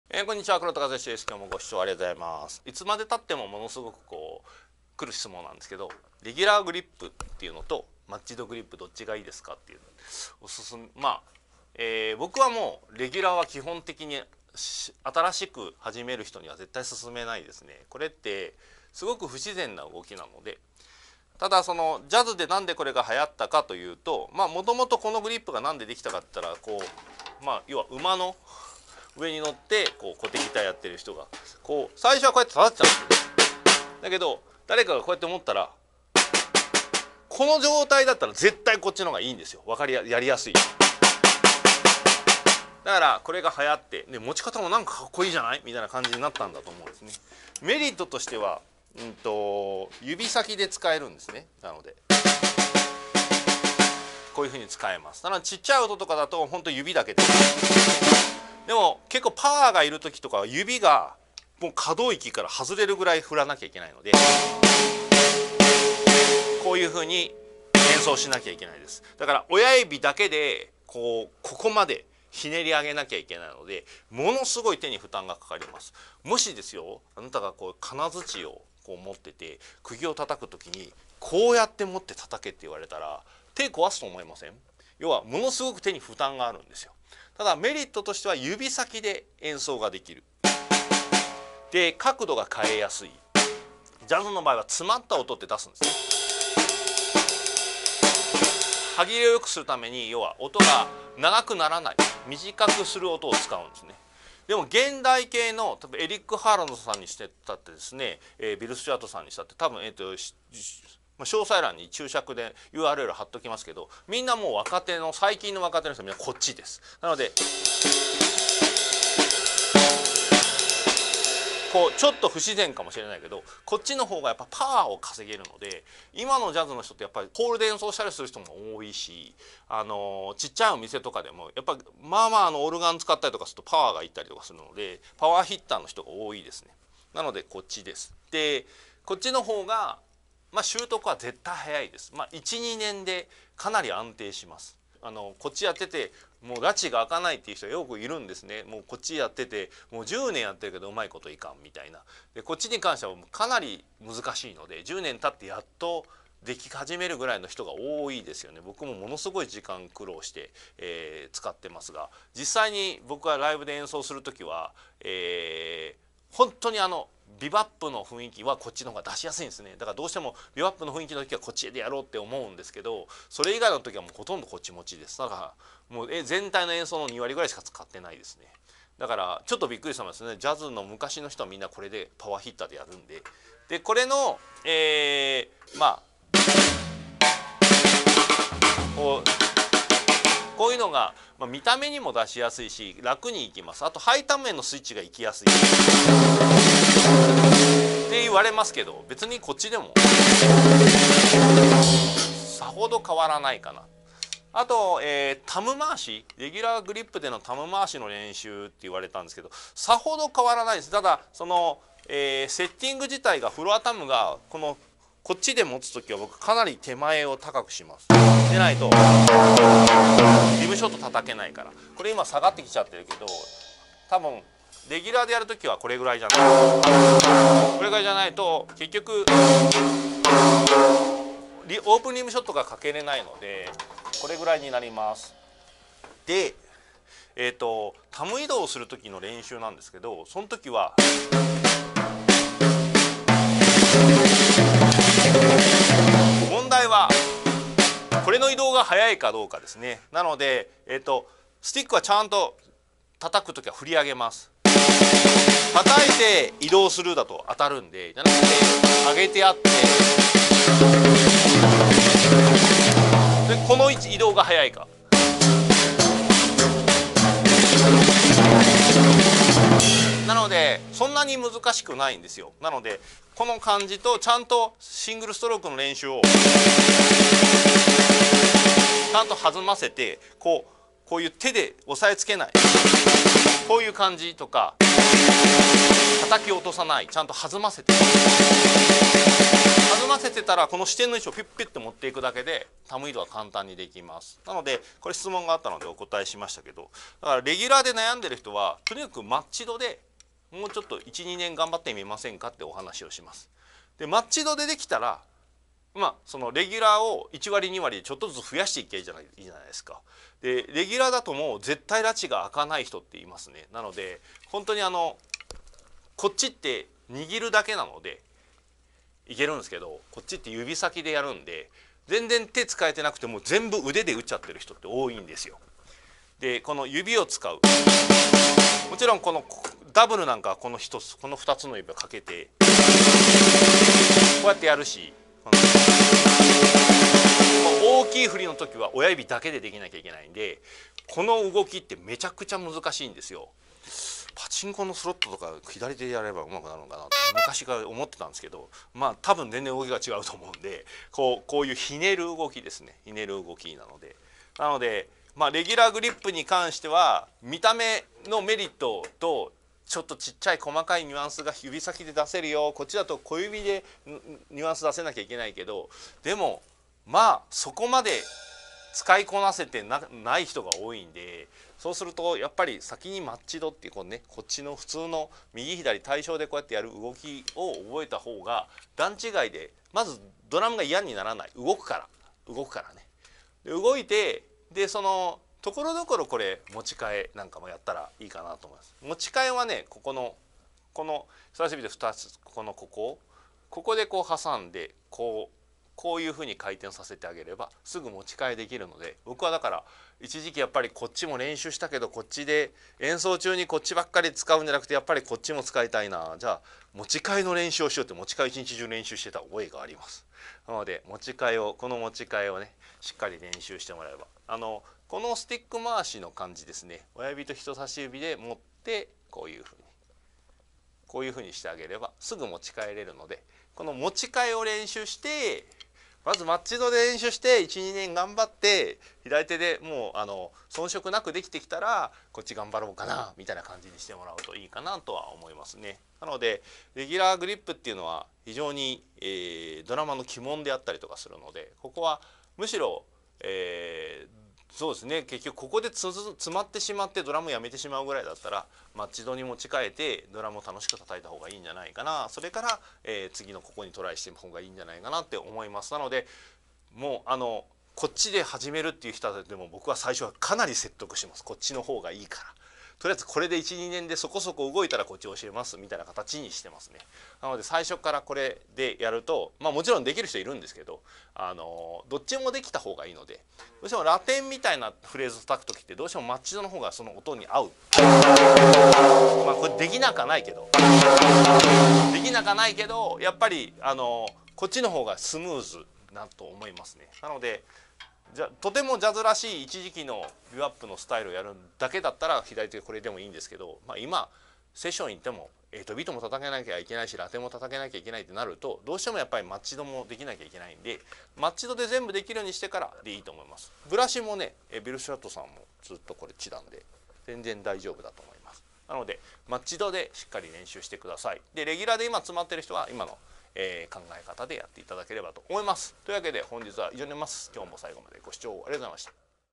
レギュラーグリップは使い難い？レギュラーのタム回し・マッチドへの持ち替え
レギュラーグリップは習得7年といわれます。
どうしてもジャズから始める人はあこがれますが、
基本おすすめできません。
これが原因で10年やっても全然うまくならない人を何人もみています。
現代の若手ドラマーはほとんどみんなマッチドです。
マッチド2年頑張ってからをおすすめします！